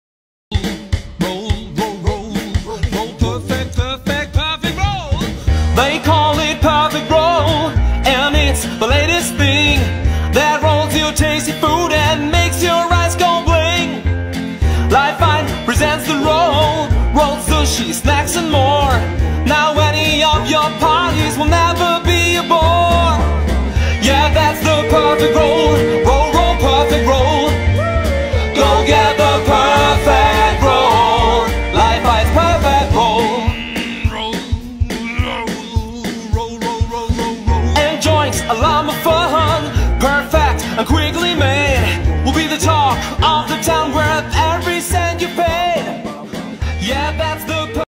Best Magic Sushi Roll Maker Machine Kitchen Gadgets Tools
Turn whatever you like into a roll whether it's fish, meat or vegetable.Invite your friends to a party and the Sushi maker will become an instant conversation piece.
This Perfect Sushi Maker will bring Asian delicacies to your kitchen. Making sushi rolls with a bamboo mat is very difficult and frustrating, this will definitely cut out.